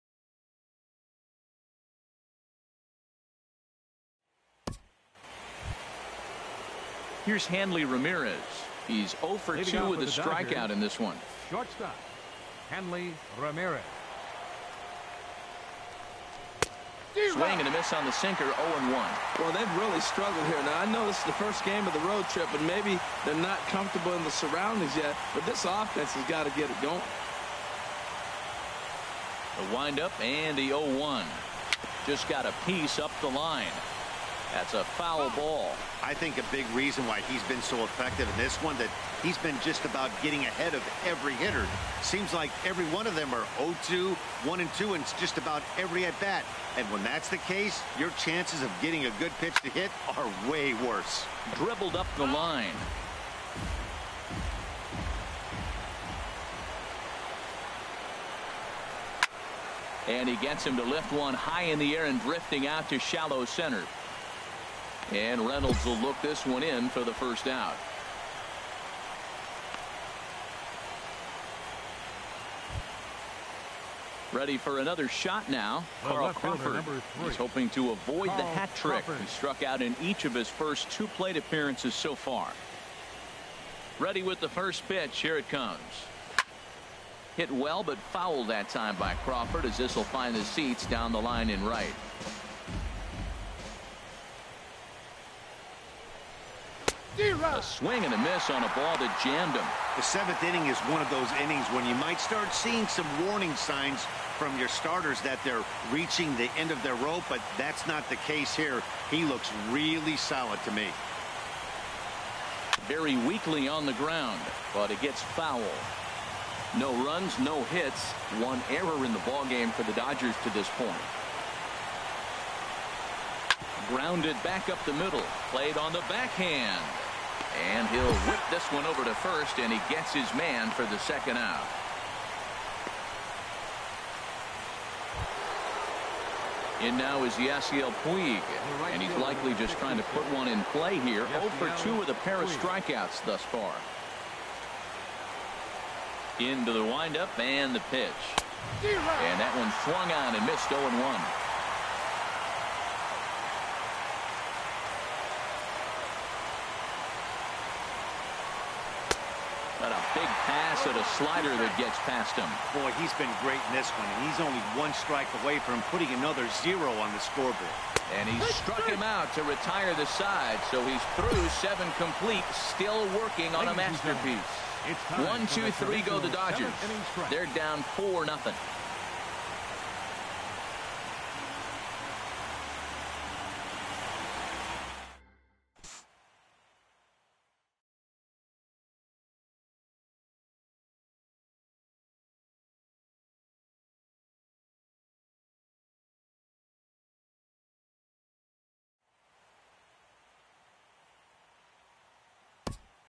The Greatest Game Ever Pitched? - World Series Game 6 - 7th Inning - Cleveland v. Los Angeles
11/2/15 - "The General" has been nearly untouchable through six innings.  He enters the seventh with fifteen strike outs, but again faces the top of the order in Hanley Ramirez, Craig Crawford and Yasiel Puig.

Will this be the inning where the magic ends?